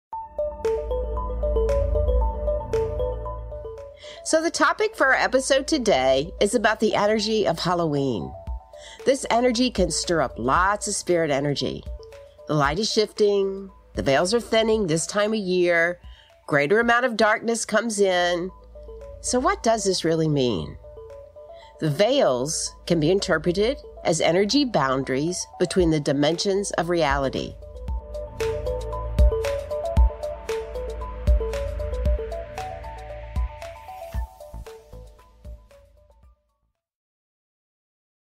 The Empowered Spirit Show - Hallloween Special
The Veils are Thinning... What Does Spirit Offer at this Time?
The topic for your episode today is about the Energy of Halloween.  This energy can stir up lots of spirit energy.
The light is shifting.
The veils are thinning this time of year.
Greater amounts of darkness fall in with the longer nights.
The veil between the living and the dead is also said to be thinnest due to the Sun’s position in the deep and intuitive sign of Scorpio.
A portal of energy unlocks at this time of year allowing for deeper connections with the spirit world to be made.
Halloween is a time to honor the “deaths” that you have experienced throughout the year. It is a time to make peace with all the things you have needed to let go of and to celebrate the cycle of the new.
Listen  to the complete episode at terriannheiman.com